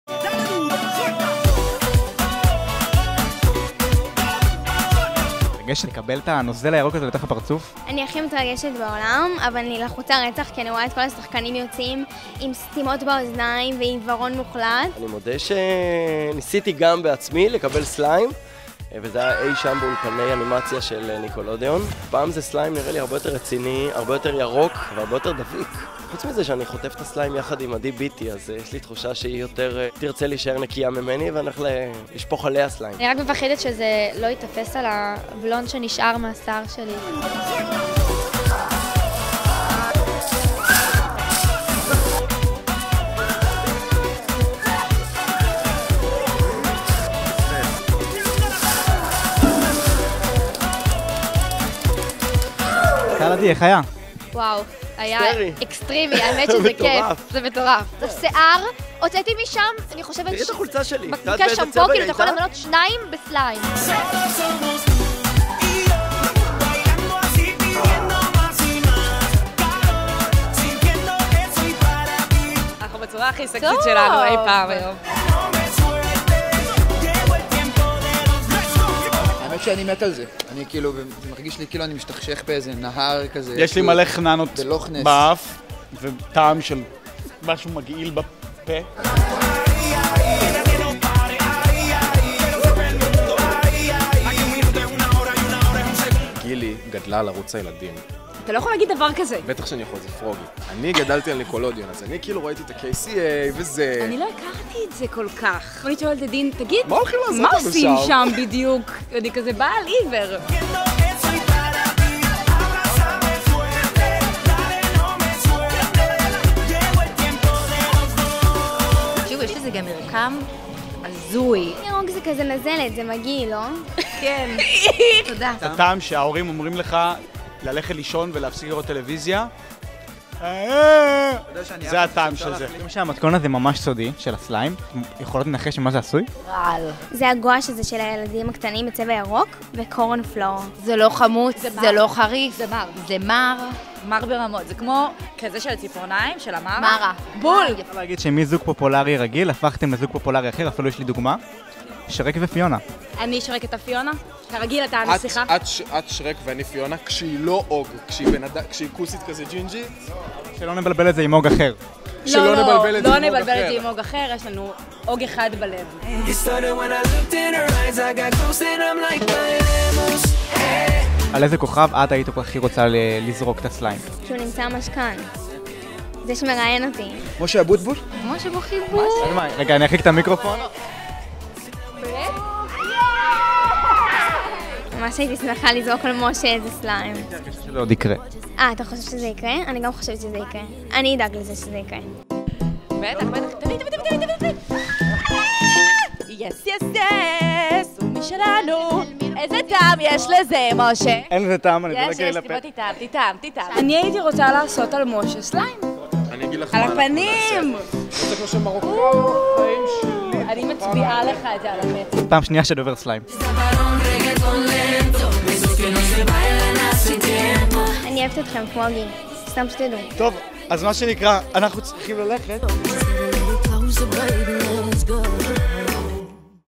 הנקמה הגדולה של כוכבי ניקלודיאון!!!
כוכבי הערוץ הכי כתום הפכו לירוקים במיוחד לכבוד צילומי סרטוני הסליים המסורתיים של הערוץ, הצטרפנו אל מאחורי הקלעים של המאורע המרגש והדביק ונאלצנו לחוות נקמה נוזלית במיוחד. צפו

אפליקציית פרוגי להורדה בחינם!